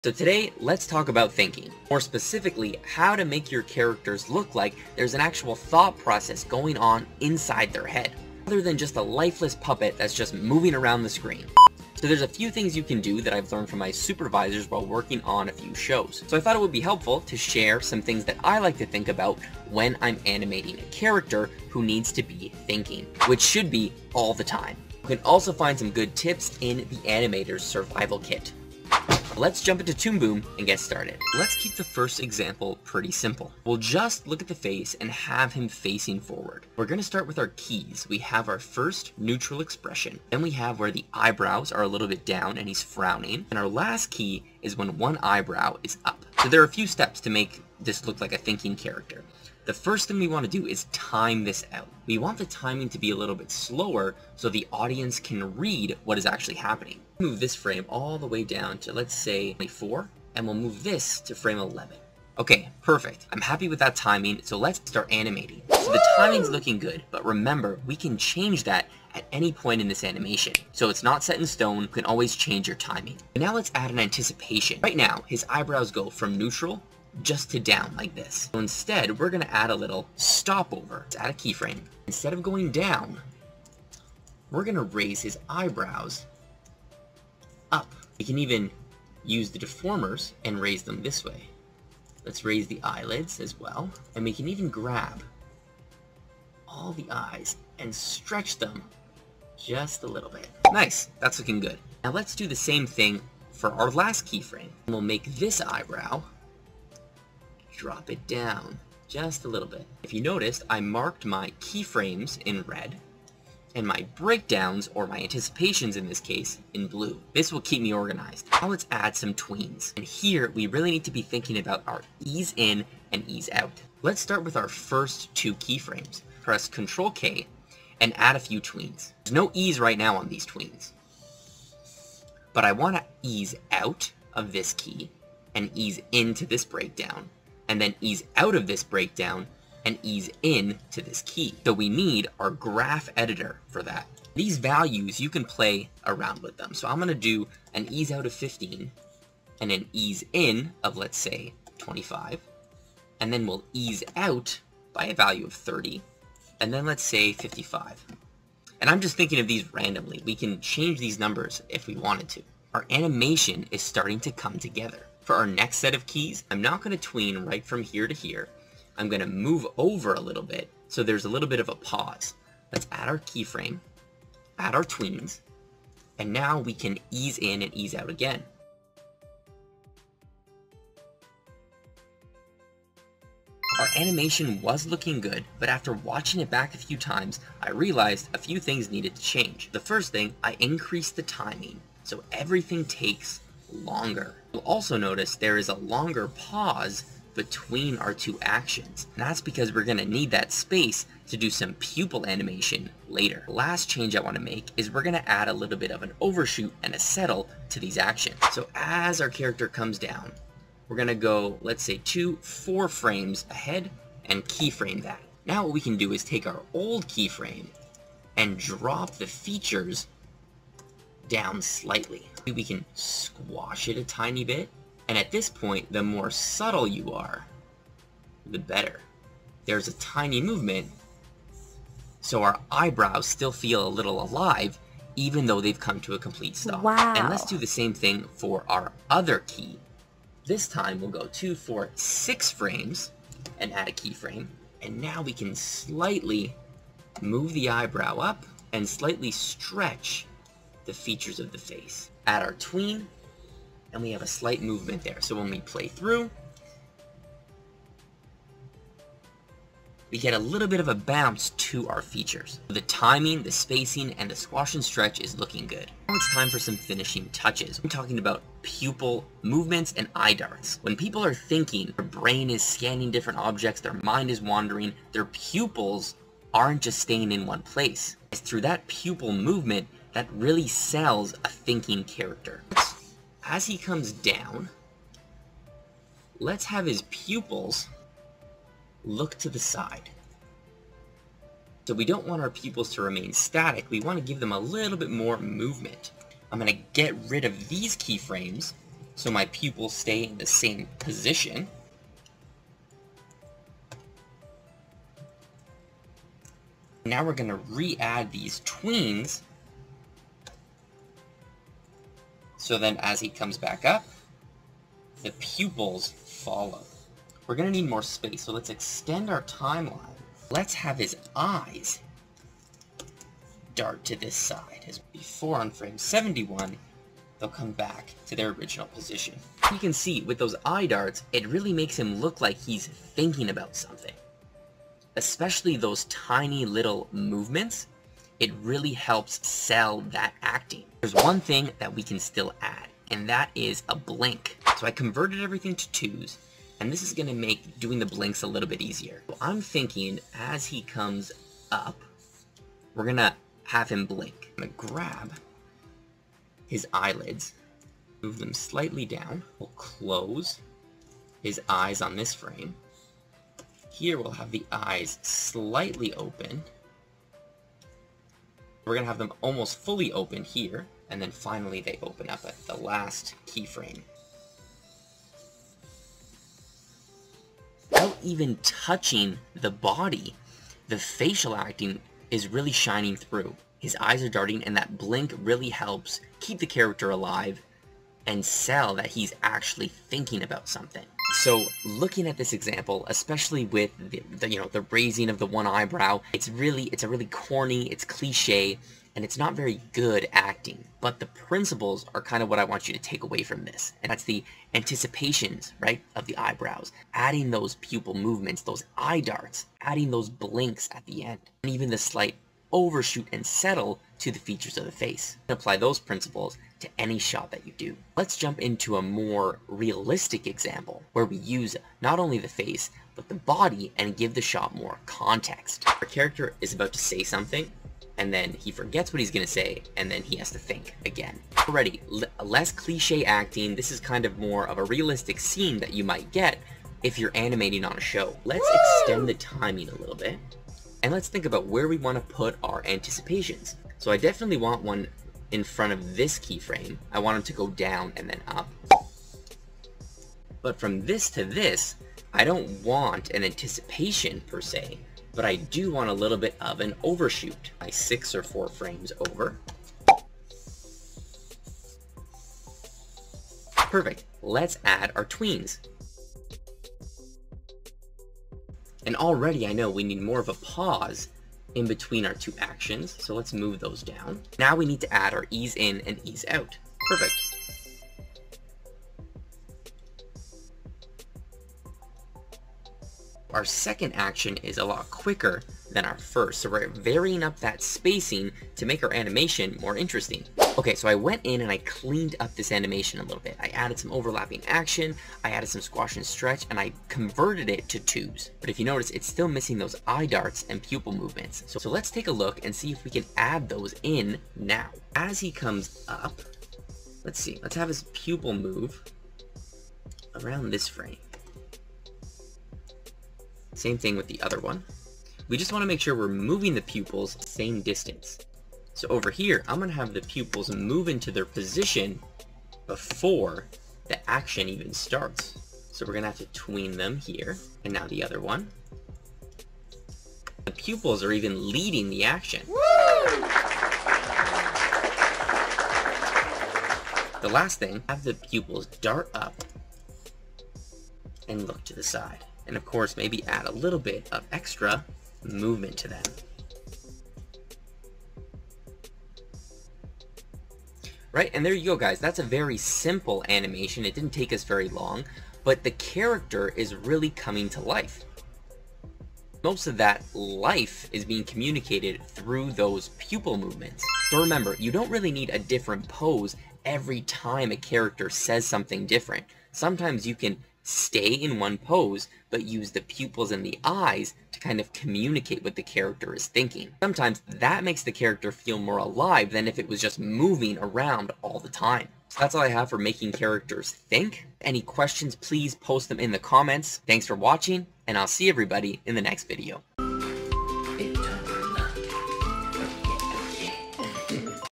How to Animate a “Thinking Character” (You Need to Start ￼Doing This)
Making characters look like they are thinking is one of the hardest things to do! Animating large movements is easy, but subtle acting can be tricky. I learned these tricks while working on Saving me and Corner Gas.  Hope they help!

Also you can get the rig in this video here: (it’s part of the bundle)

0:00 Thinking = Life
0:31 Example 1: Keys & Timing
1:01 Example 1: Breakdowns & Spacing
3:03 Example 1: Overshoots & Settle
4:25 Example 1: Pupil Movements
5:41 Example 1: Adding a Blink
6:31 Example 1: Takeaways
6:58 Example 2
7:21 Example 2: Breakdowns & Spacing
8:12 Example 2: Pupil Movements & Blinks
9:08 Example 2 Final Result
9:25 Final Thoughts
9:50 Marloooooo